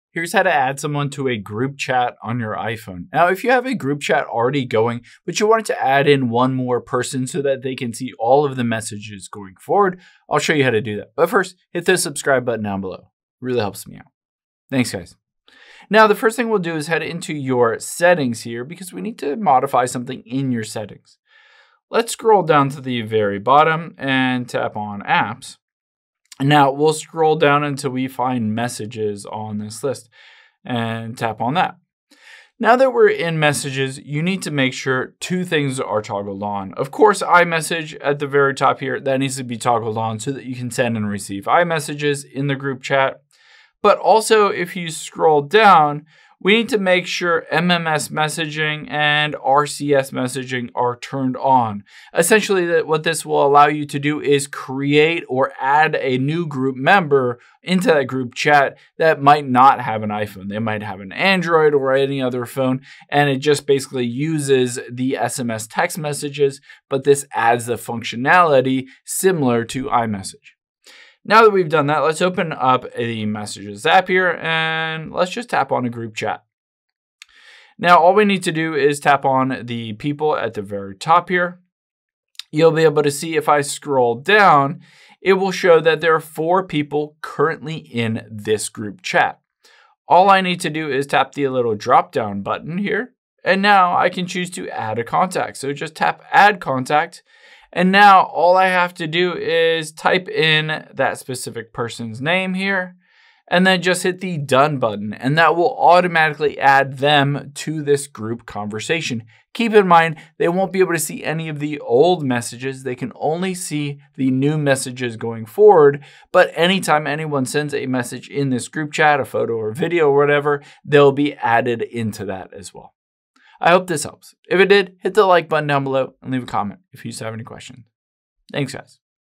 How To Add Someone To Group Chat On iPhone
Here's how to add someone to a group chat on iPhone so you can include other contacts into an existing group.